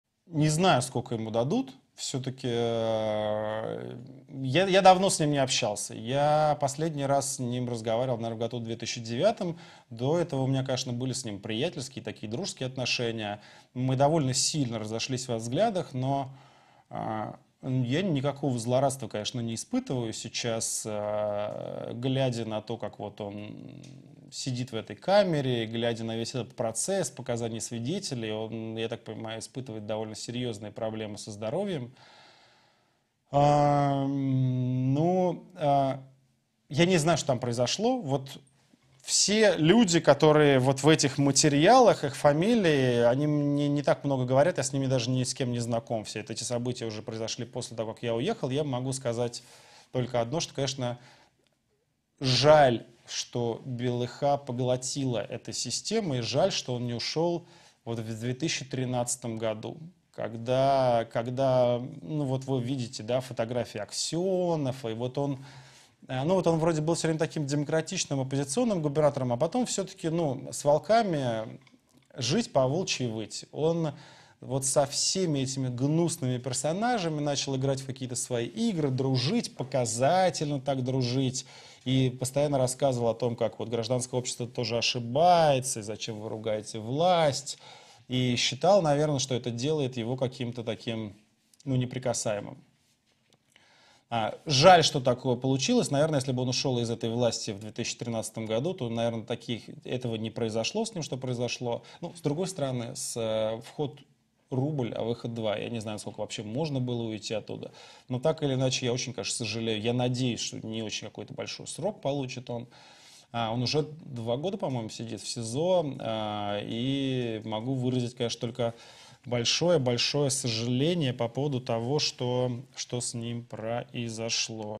Навальный - "Никита Белых был поглощен этой системой"
🌃 Алексей Навальный о суде над бывшим губернатором Кировской области Никите Белых. (Во время записи этого видео приговор был еще неизвестен)